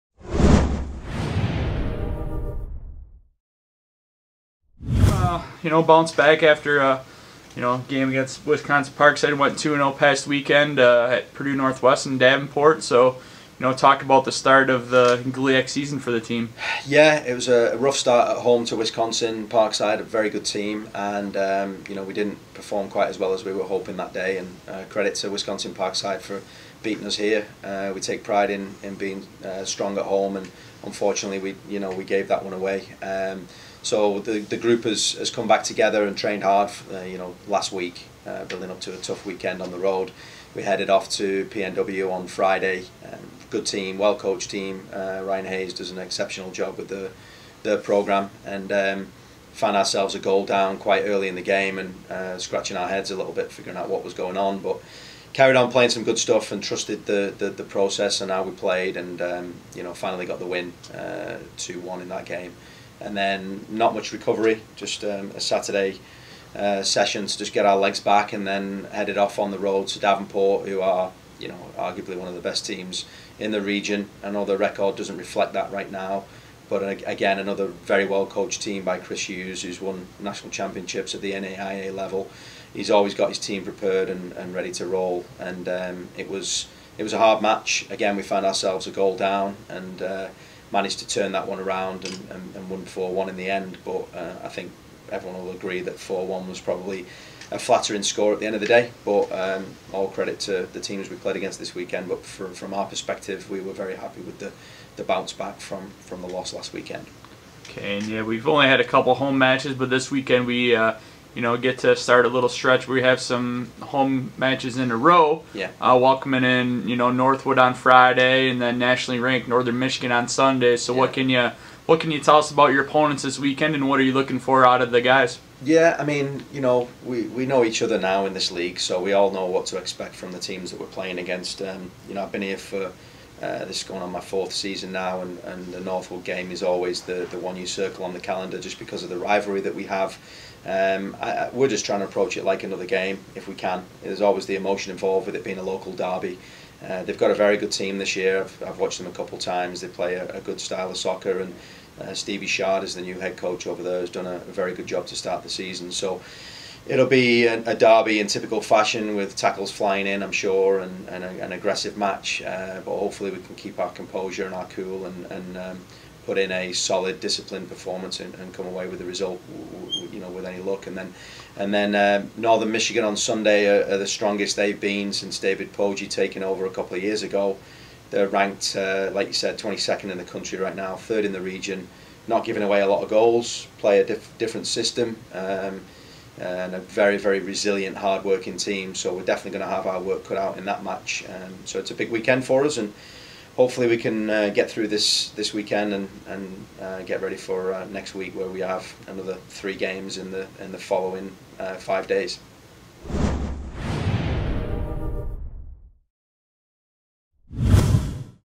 Men's Soccer Weekly Interview - PNW / Davenport
Andy Wagstaff discusses the start to the team's GLIAC slate of matches...